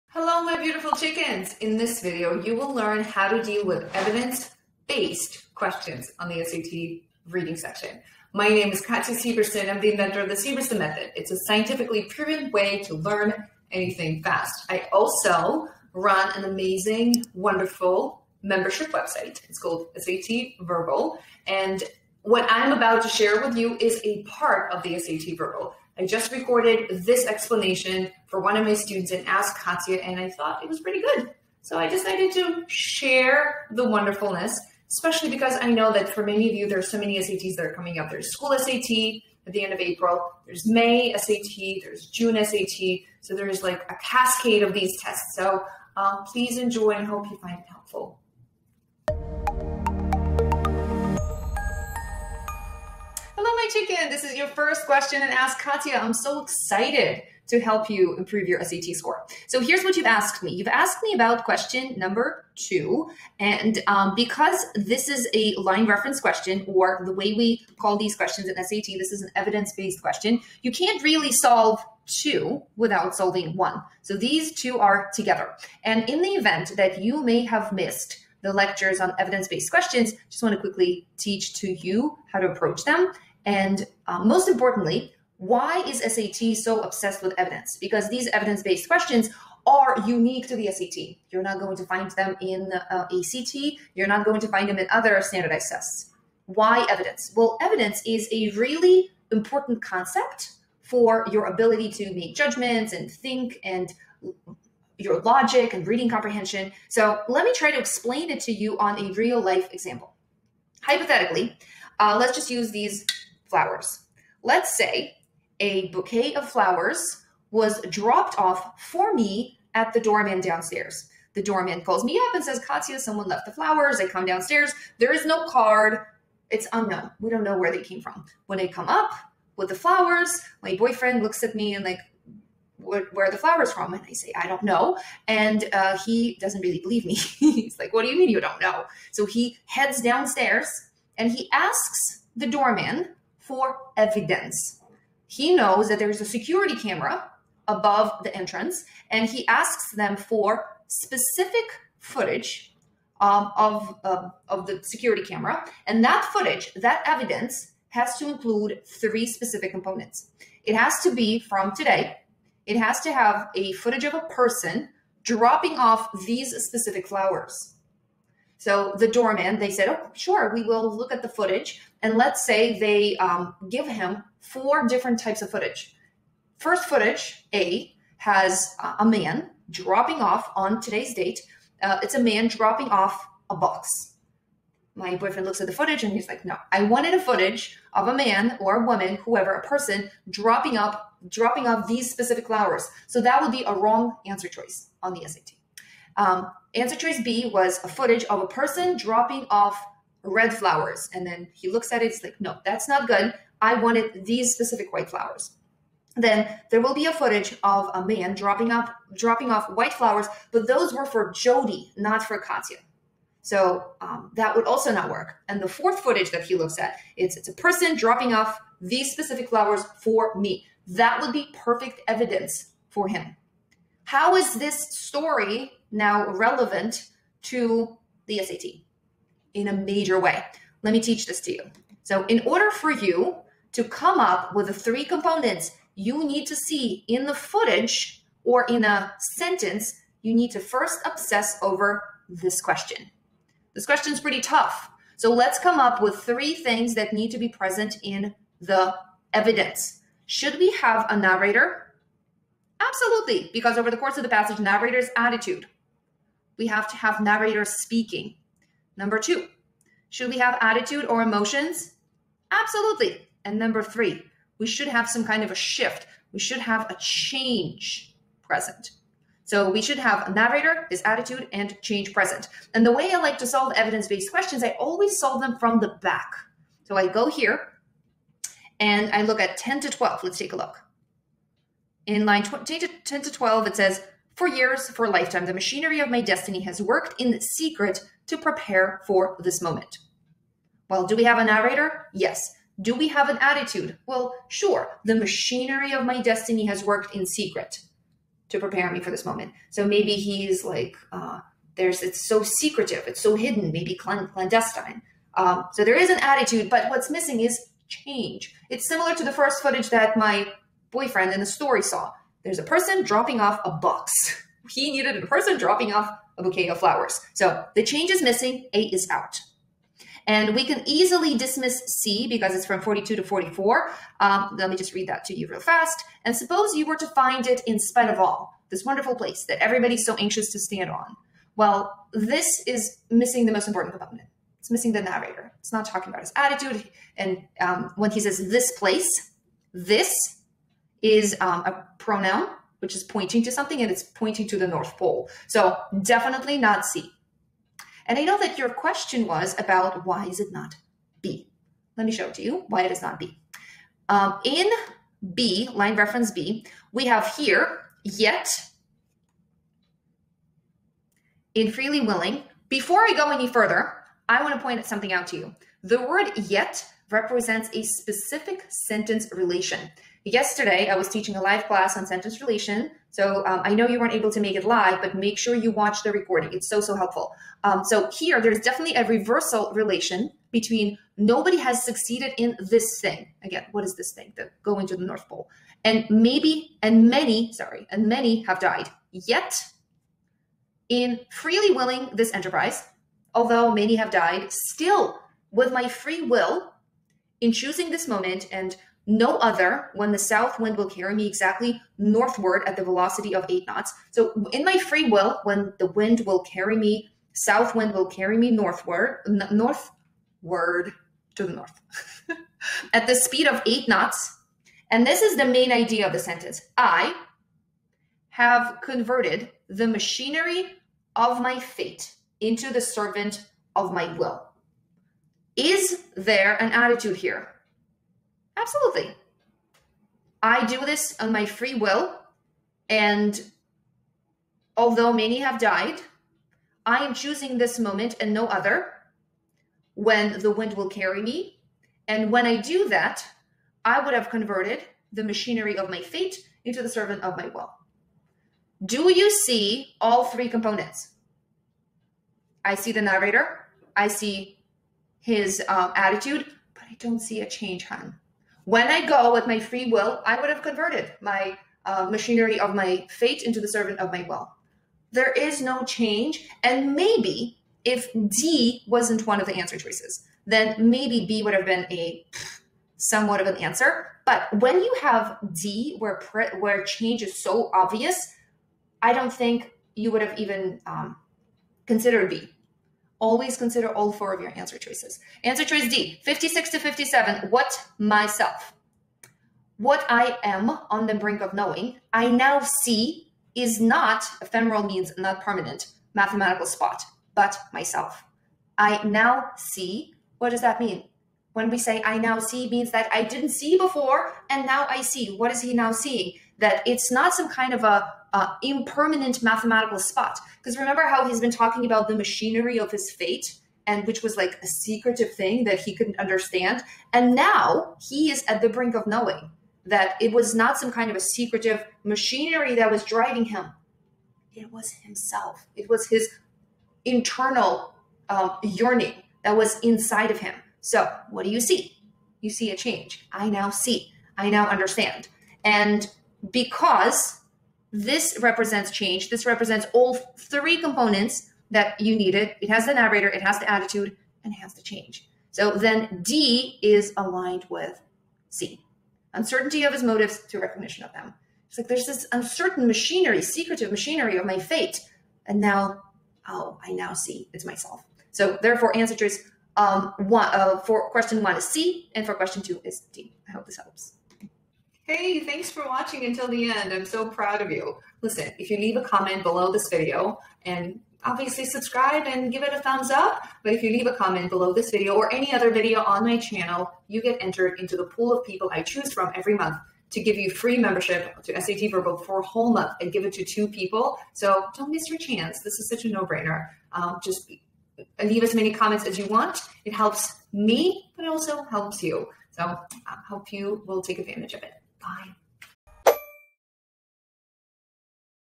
SAT Reading Tips: Evidence Based Questions ✳️.
In this video, you will learn about evidence-based questions on the reading section of the SAT test. SAT reading section has a unique obsession with contextual evidence and learning how to deal with such problems can have a tremendous impact on your SAT score.

📘To buy my speed reading book